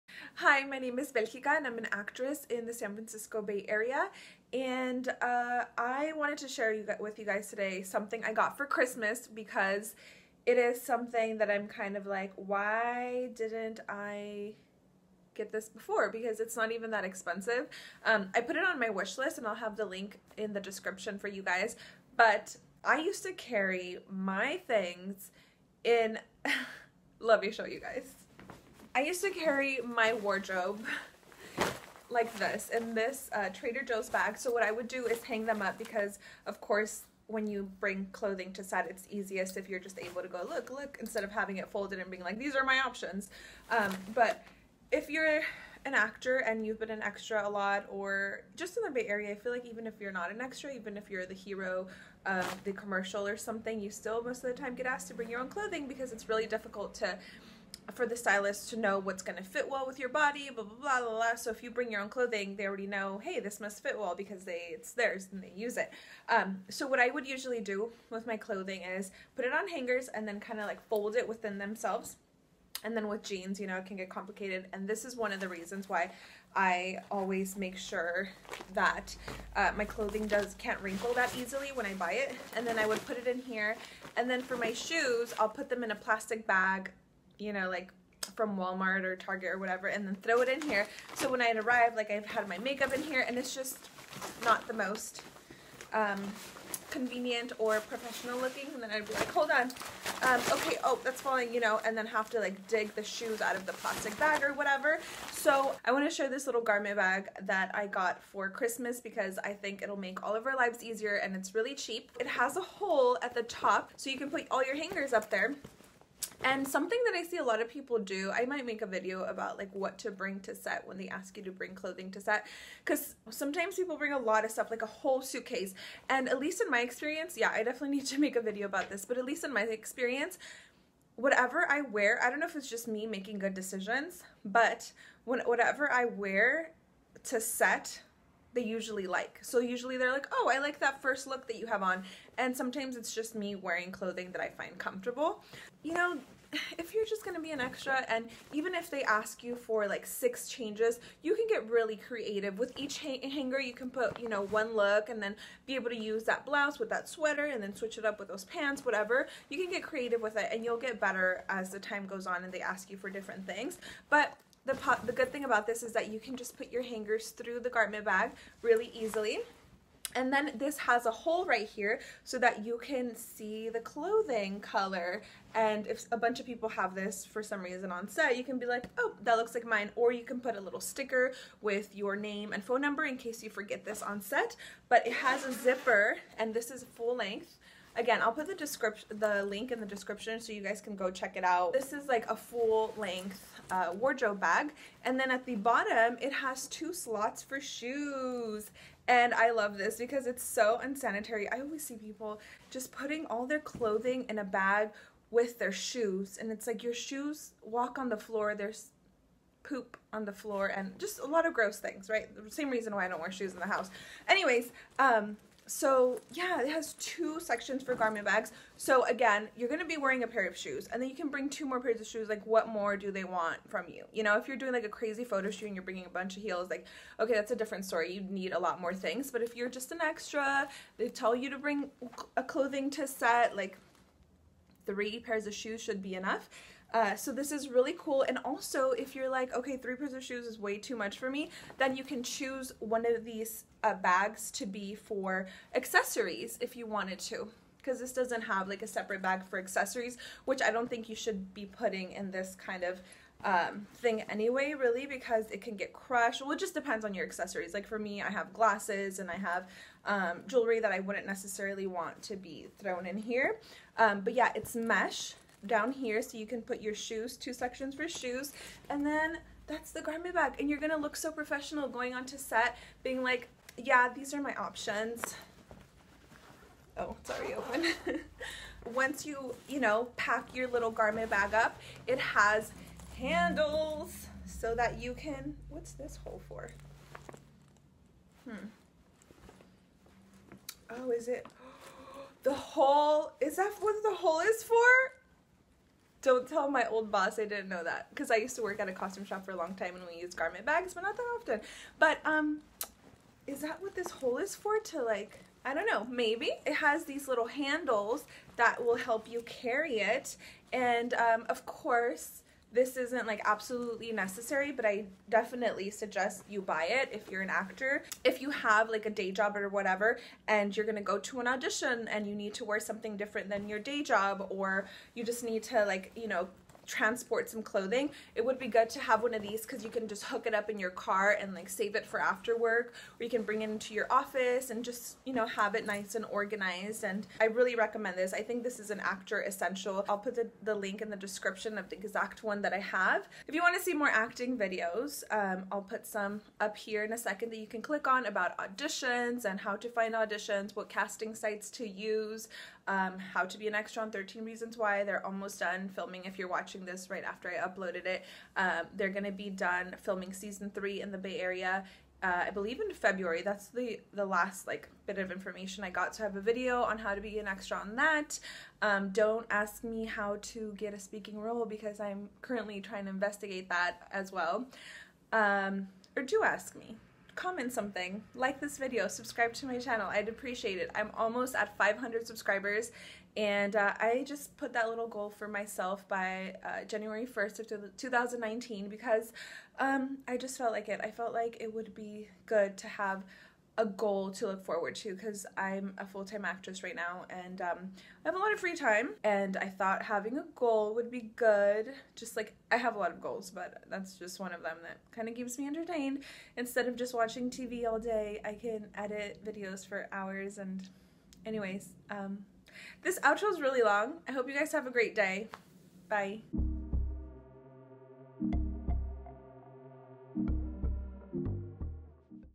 The Perfect Garment Bag | Actor Essentials | Amazon
Garment Bag | Actor Essentials  | Amazon
The perfect garment bag to bring to set!
How to bring your clothing to set in a professional way!

As an actress, I always get asked to bring my own clothing to set and now I can do it in a sanitary, organized way! I am so excited that I got this and now can easily carry my wardrobe!